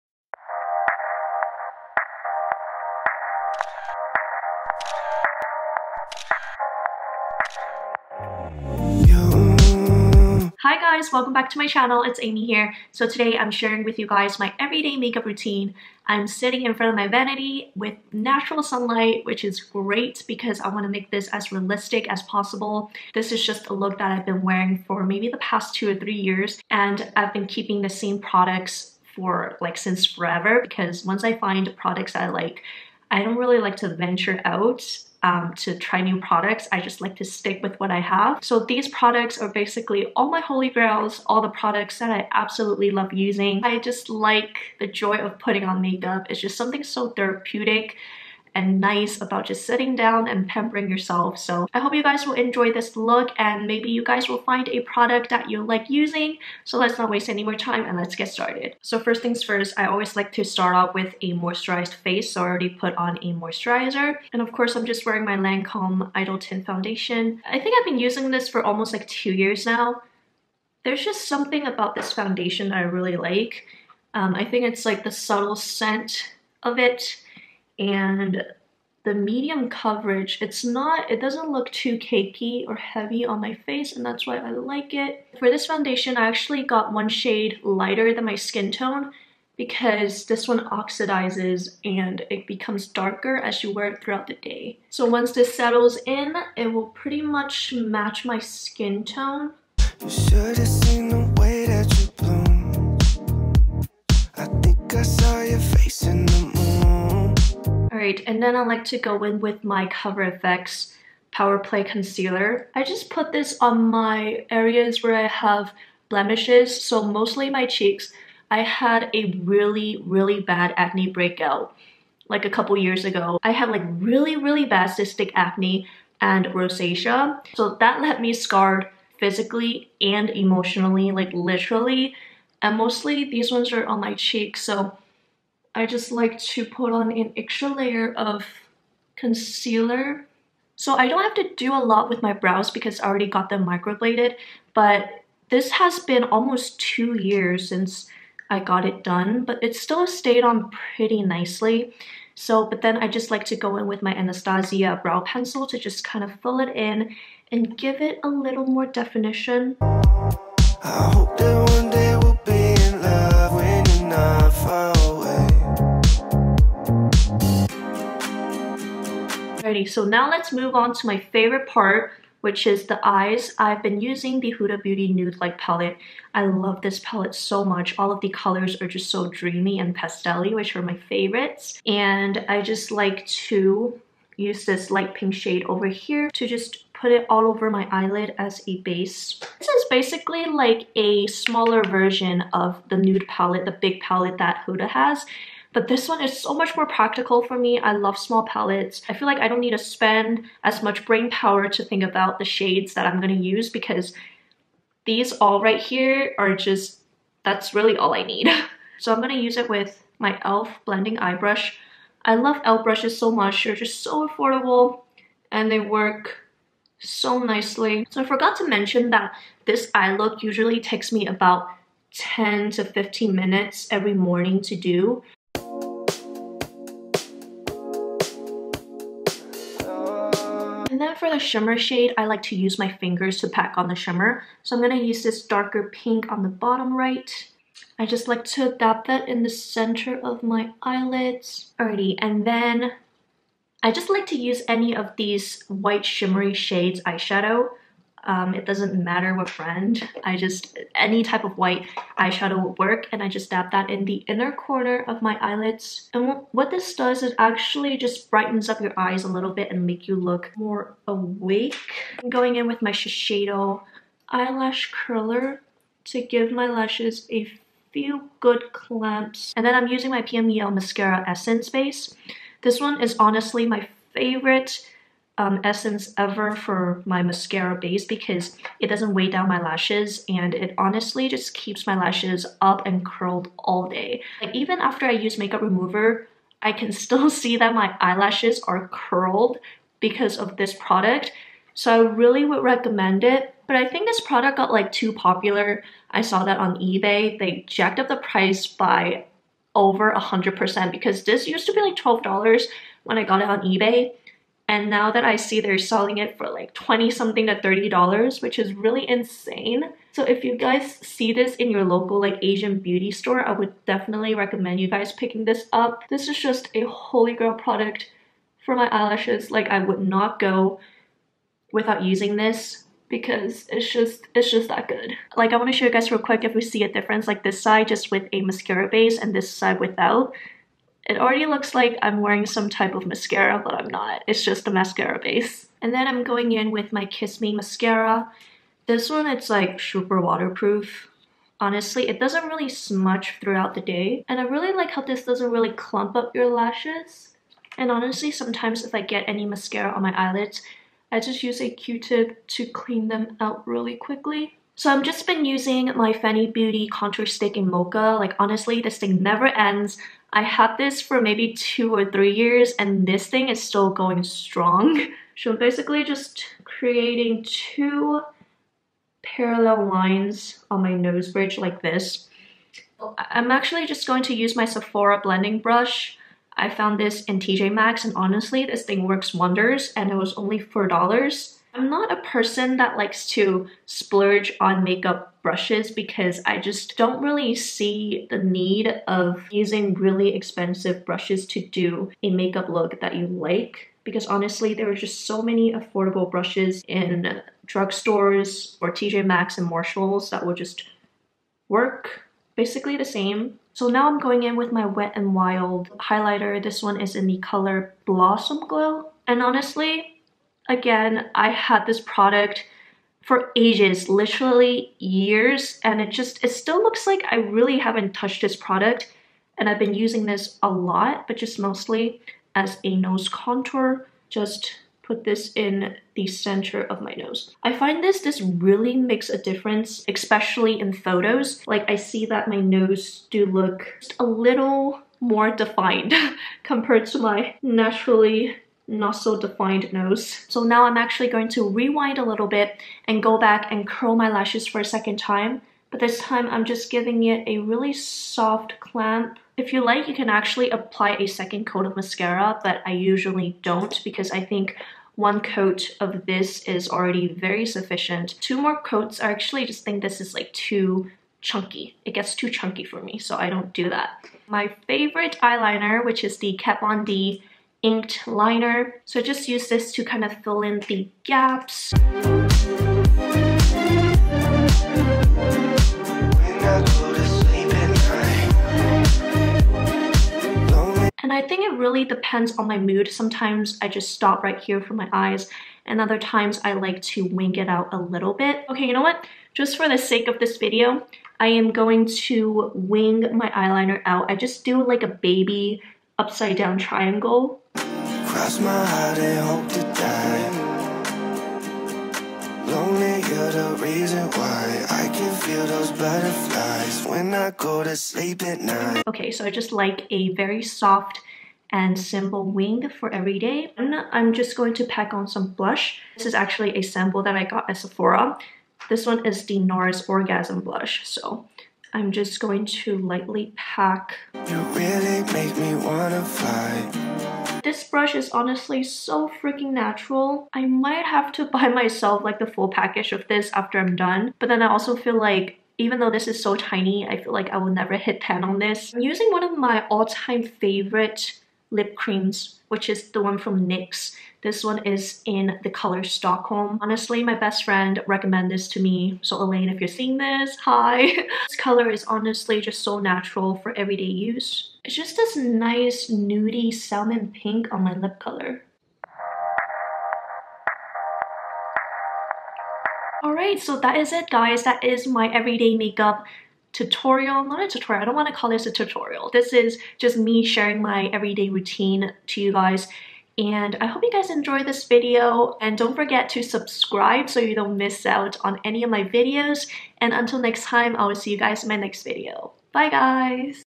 My Everyday Makeup Routine 2021 || Quick & Simple
Hi friends! Here is my everyday makeup routine. When I said this "eye look" takes 10-15 mins to do, I really meant this whole makeup look only took me 10-15 mins to do! It's that quick and simple

Products Mentioned:
Lancome Teint Idole Ultra Wear Foundation in 100 Ivory N
Cover Fx Power Play Concealer
Huda Beauty Nude Light Palette
CT Pillow Talk
PMEL eyelash mascara base
Kiss me Heroine Make long and curl mascara
Kat Von D Ink eyeliner
Anastasia Brow Definer - medium brown
Fenty Beauty Contour stick - Mocha
Wet n wild highlighter - blossom glow
Nars Orgasm Blush
Nyx soft matte lip cream - Stockholm